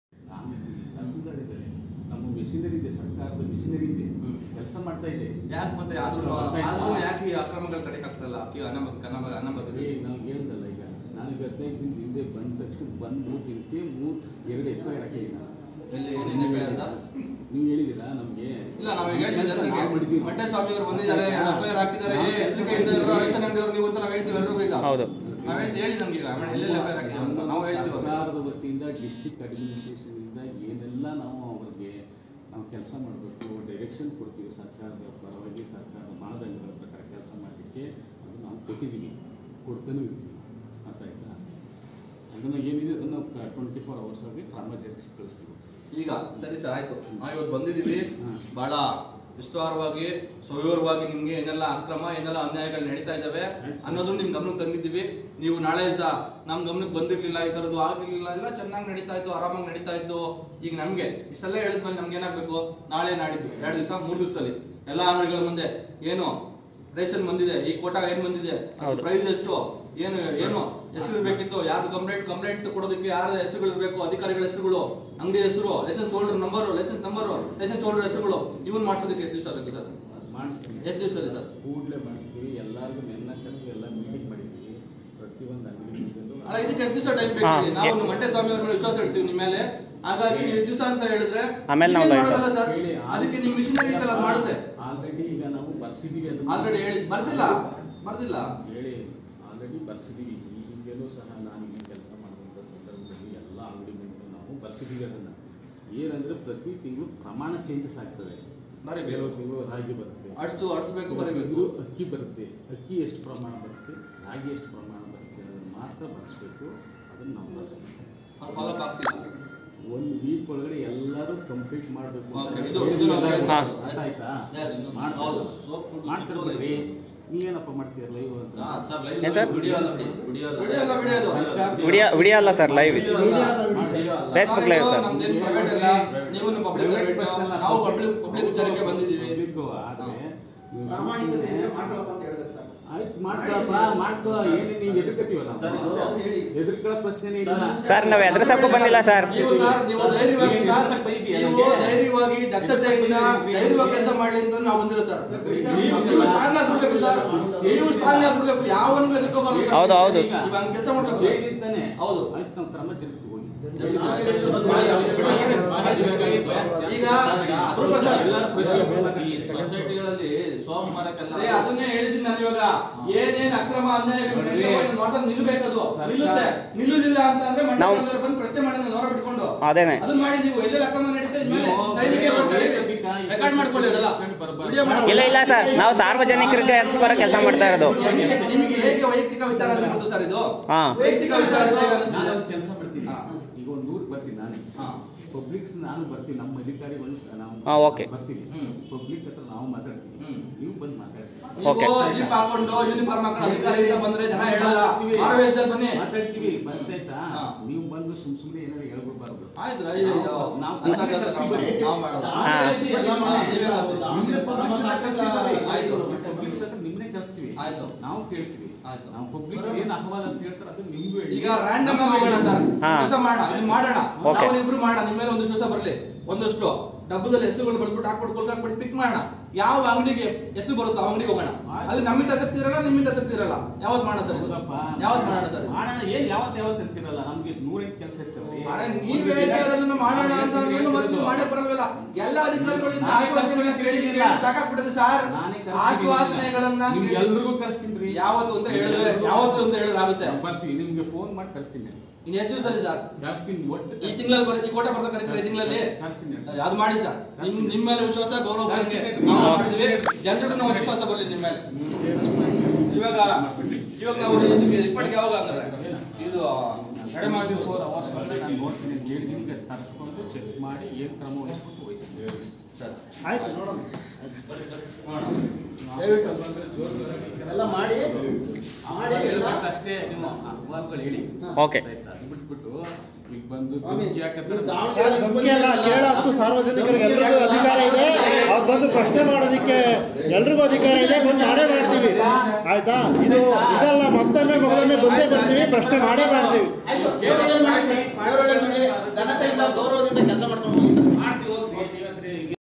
ಅನ್ನಭಾಗ್ಯ ಯೋಜನೆಯ ಪಡಿತರ ವಿತರಣೆಯಲ್ಲಿ ನ್ಯಾಯಬೆಲೆ ಅಂಗಡಿಯವರು ನಡೆಸುತ್ತಿರುವ ಮೋಸ ಮತ್ತು ಅಕ್ರಮಗಳು ಬಗ್ಗೆ-Part-2
ಅನ್ನಭಾಗ್ಯ ಯೋಜನೆಯ ಪಡಿತರ ವಿತರಣೆಯಲ್ಲಿ ನ್ಯಾಯಬೆಲೆ ಅಂಗಡಿಯವರು ನಡೆಸುತ್ತಿರುವ ಮೋಸ ಮತ್ತು ಅಕ್ರಮಗಳು ಬಗ್ಗೆ ದೂರು ನೀಡಿದರೂ ಕ್ರಮ ತೆಗೆದುಕೊಳ್ಳದ ಆಹಾರ ನಾಗರೀಕ ಸರಬರಾಜು ಇಲಾಖೆ ತುಮಕೂರು ಜಿಲ್ಲಾ ಜಂಟಿ ನಿರ್ದೇಶಕರನ್ನು ಪ್ರಶ್ನೆ ಮಾಡುತ್ತಿರುವ ಕೆ.ಆರ್.ಎಸ್ ಸೈನಿಕರು.